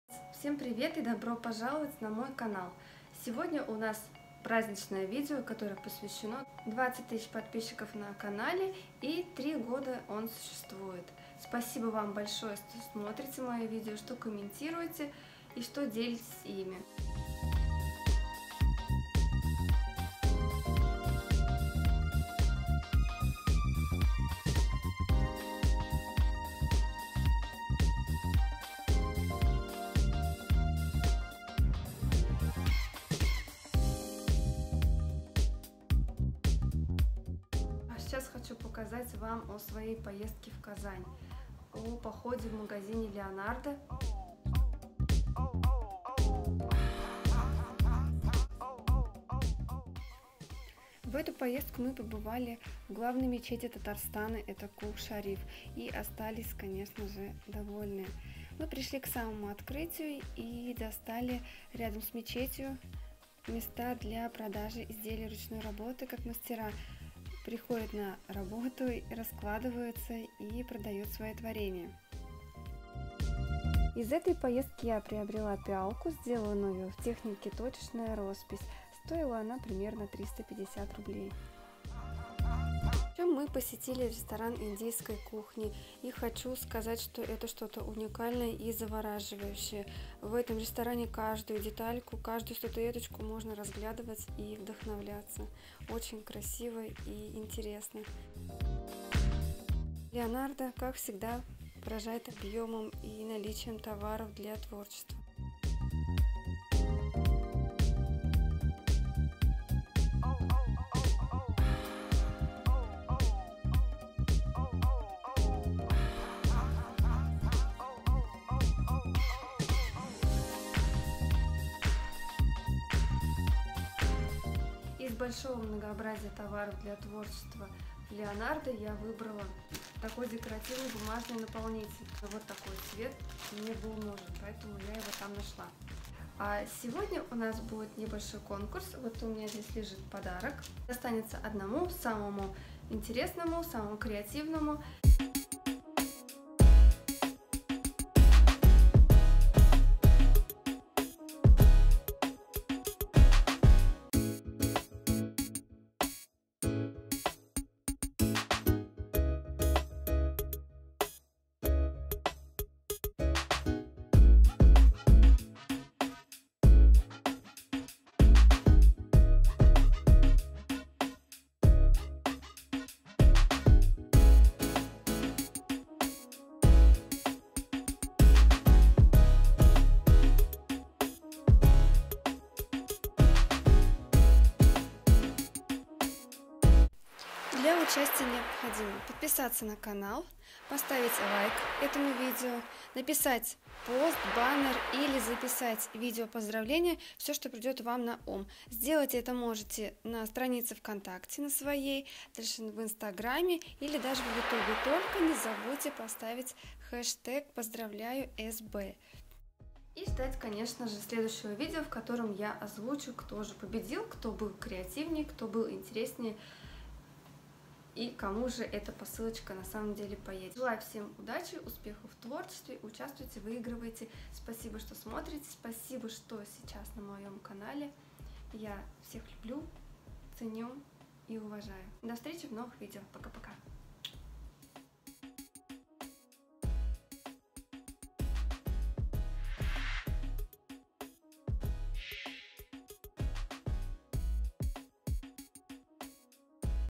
Vlog ♥ 20K ♥ Конкурс для подписчиков ► Скрапбукинг Scrapbooking
На своей станице (контакт или инстаграм или ютуб) написать пост, выложить фото или записать видео поздравление.
Хештег поздравляю_SB ✔
Комментарии к виде тоже участвуют в конкурсе!
В общем поздравить креативно и интересно!!! ✔

Самые интересные работы попадут в следующее видео и даже инстаграм.
Конкурс для тех, кто не боиться показать себя и найти новых зрителей!

Если вы являетесь производителем товаров для творчества или у вас есть свой магазин и вы готовы сотрудничать и развиваться, пишите в личку